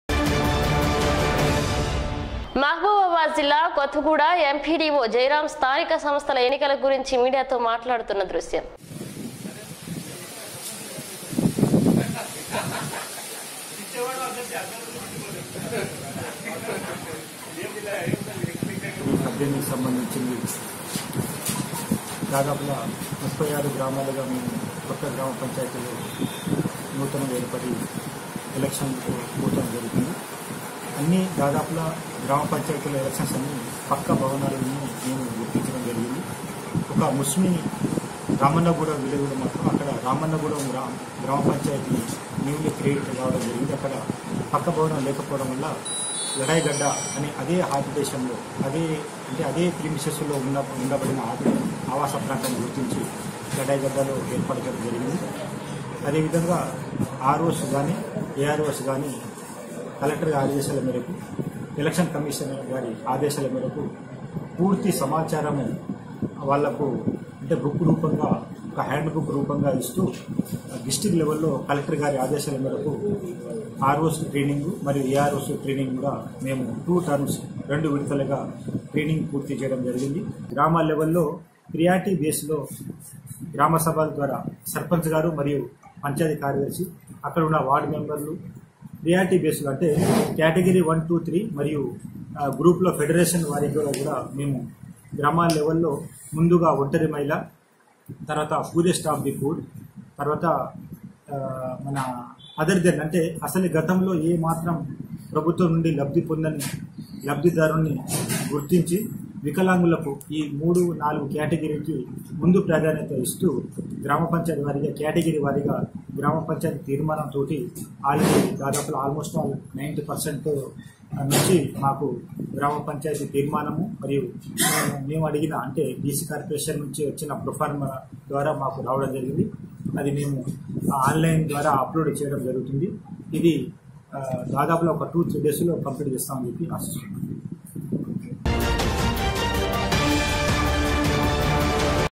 మహబూబాబడి జిల్లా కొత్తగూడ M.P.D.O జయరాం స్థానిక సంస్థల ఎన్నికలకు గురించి మీడియాతో
మహబూబాబడి జిల్లా కొత్తగూడ M.P.D.O జయరాం స్థానిక సంస్థల ఎన్నికలకు గురించి   మీడియాతో మాట్లాడుతున్న దృశ్యం.